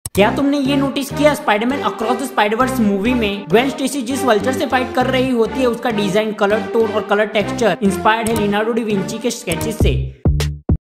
Spiderman across the spiderverse movie vulture detail 😱
Spiderman across the spiderverse movie vulture detail and fact video shorts in hindi by iroAs cinetalks

Leonardo di Vinci Easter egg